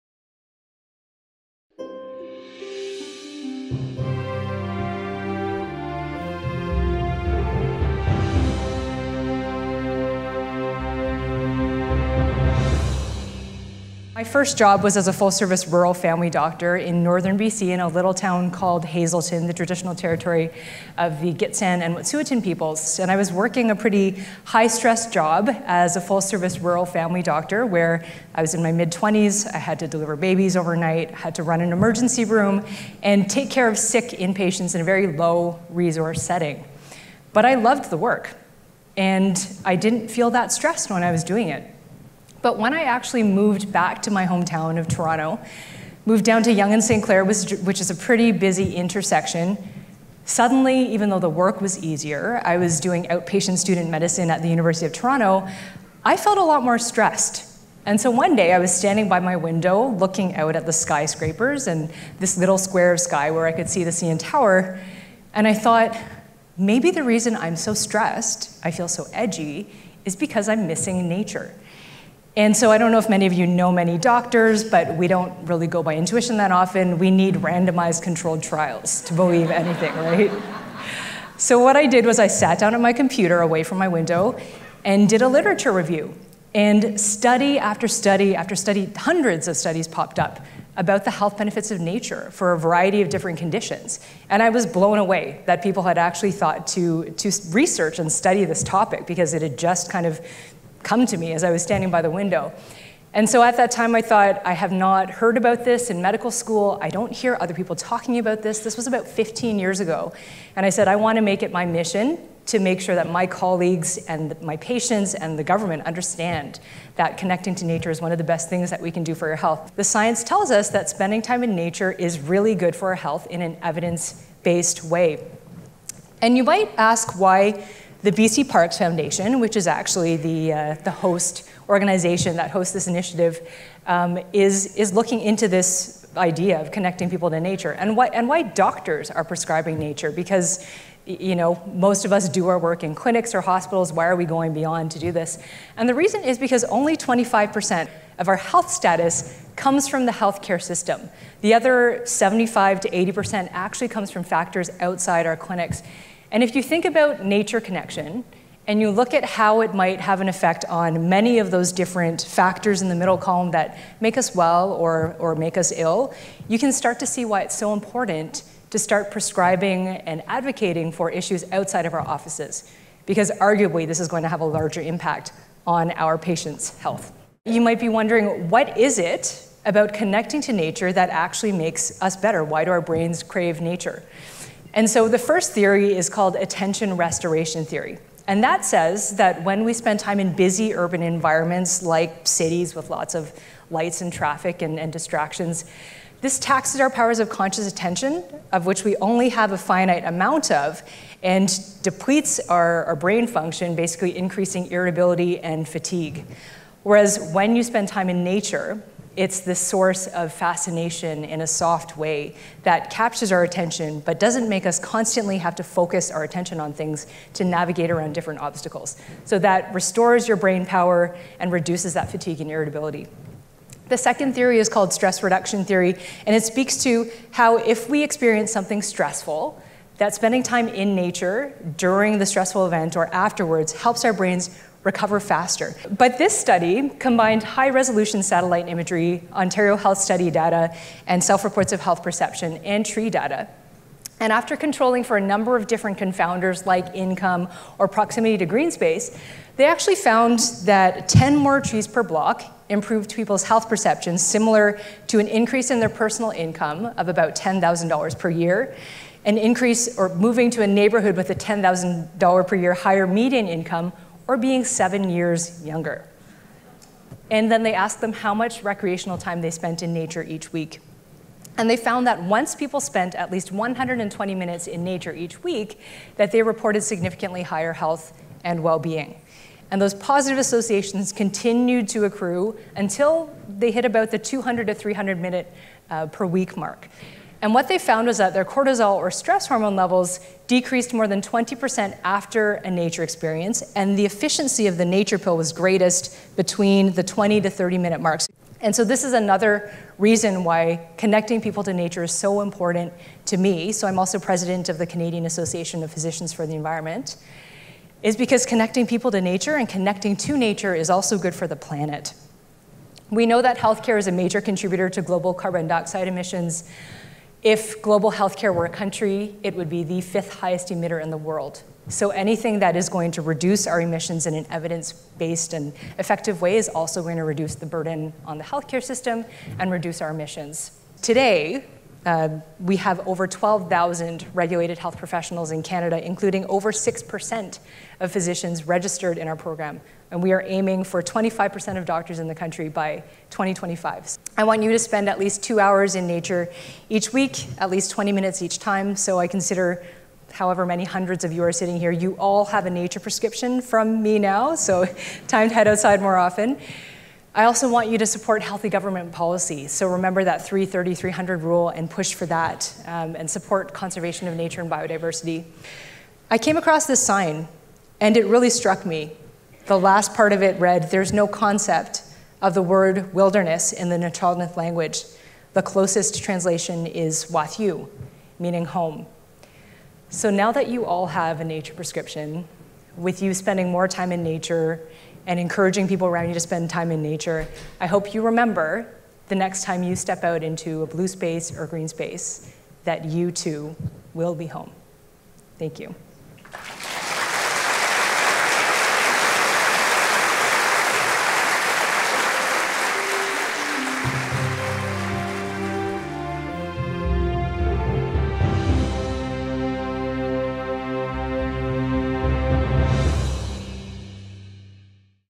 DSS Speaker Dr. Melissa Lem Abridged Talk: PaRx: A Prescription for Personal and Planetary Health
As part of UBCO’s Irving K. Barber Faculty of Arts and Social Sciences’ Distinguished Speaker Series, Dr. Melissa Lem discussed highlights from the wide-ranging body of knowledge on nature and health, presented an overview of PaRx, including its significant national and international influence, and revealed how prescribing nature can improve both patient and planetary health.